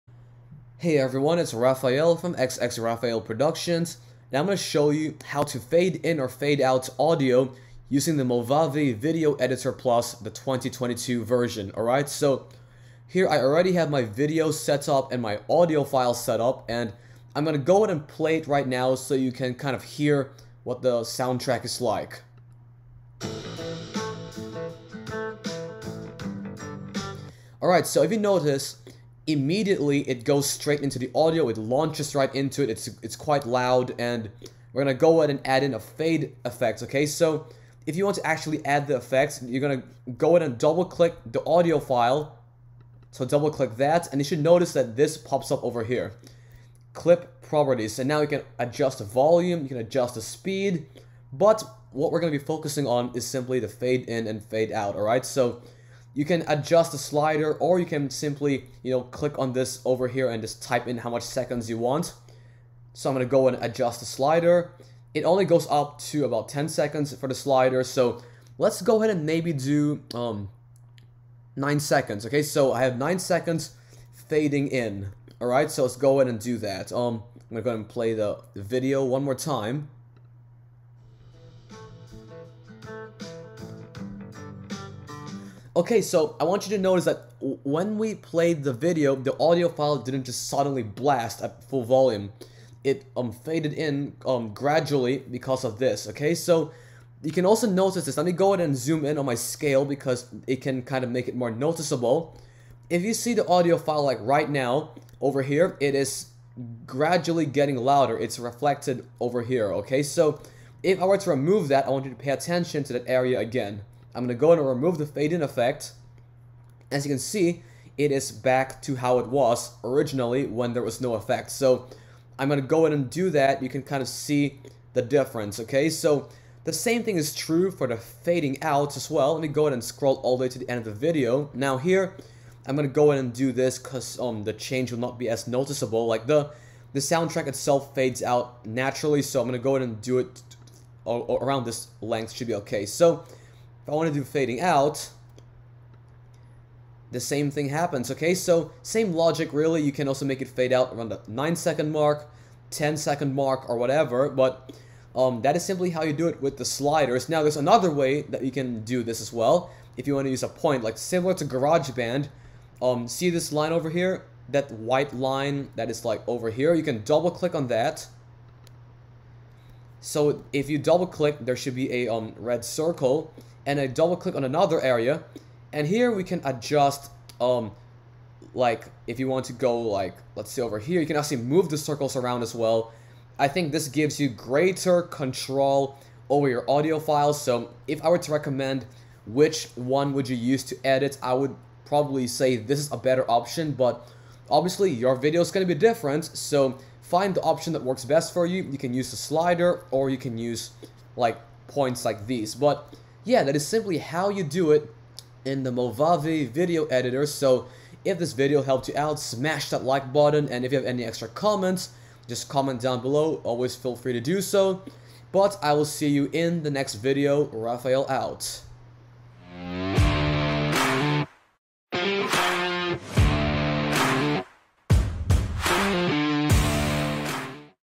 How to Fade in or Fade Out Audio using the Movavi Video Editor Plus | 2022
A simple tutorial on how to fade audio/music in and out in Movavi Video Editor Plus 2021 or 2022.  There are two ways to do it, and I explain my preferred choice by the end of the video. I hope this helped you out!

SKYPE: rafa.v35